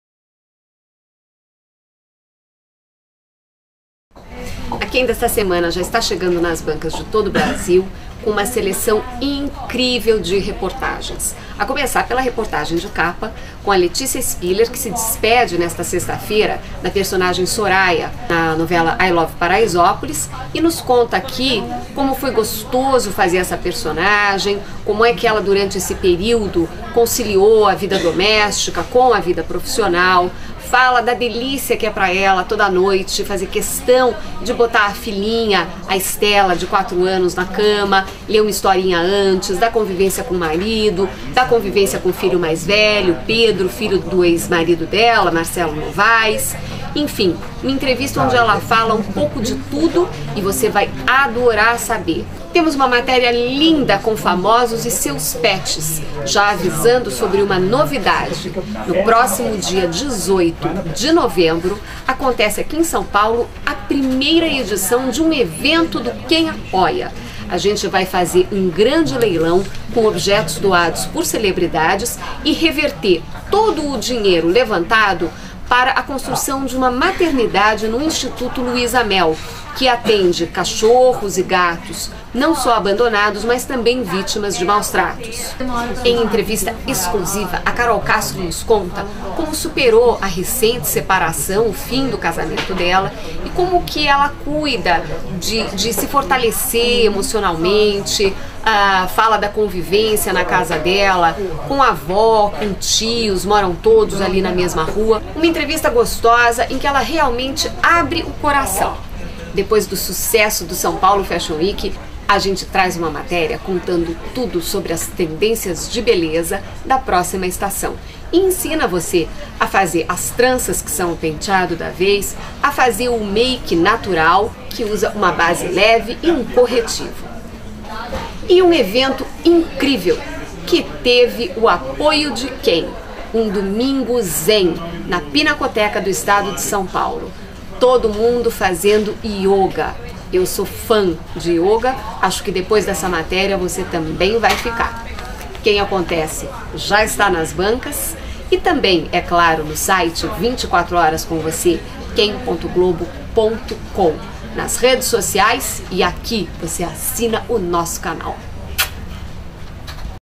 Letícia Spiller é capa de QUEM da semana
A diretora de redação de QUEM Lucia Barros conta o que tem de mais especial na revista desta semana. Letícia Spiller, que se despede de I Love Paraisópolis, é a capa e fala sobre trabalho, maternidade e planos de aumentar a família. A edição traz ainda famosos com seus pets, tendências de beleza pós-SPFW, uma entrevista exclusiva com Carol Castro e a cobertura de um evento superespecial de yoga! Não perca!